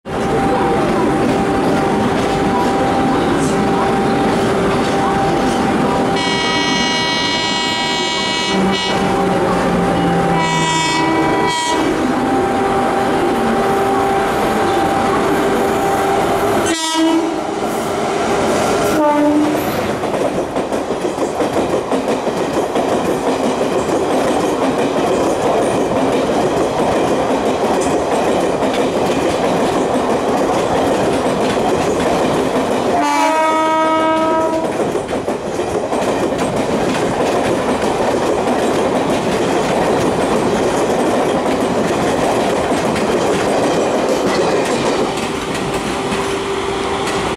12289 Mumbai-Nagpur Duronto Express Overtakes 12111 Amravati Superfast Express @ Dadar Station!!
KYN WCAM-3 hauling 12289 Mumbai CST-Nagpur Duronto Express furiously overtakes 12111 Mumbai CST-Amravati SF Express led by KYN WCAM-3 @ Dadar station of CR.